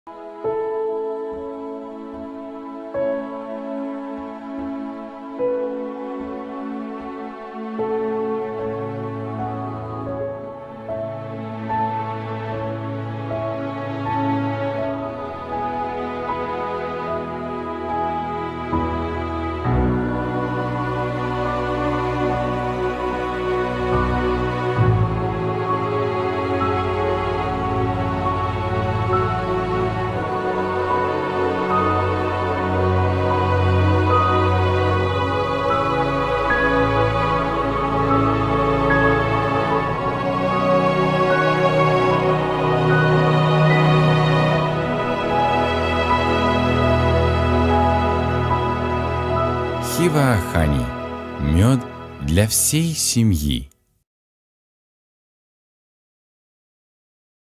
Xiva Honey - натуральный мёд для всей семьи!
"Xivahoney" - натуральный органический мёд произведенный в Узбекистане. Наш мёд взят прямо из ульев, собран пчелами из нектаров диких горных цветов в Зааминских горах Узбекистана, и является 100% натуральным.
Наш мёд генетически не модифицируется и не обрабатывается каким-либо образом, что помогает сохранить его активные, полезные свойства и натуральные компоненты. Наш мёд не смешивается с другими сортами мёда, у него неповторимый вкус.
Мы экспортируем наш органический мёд во многие страны мира! Мы с удовольствием сотрудничаем с оптовыми покупателями!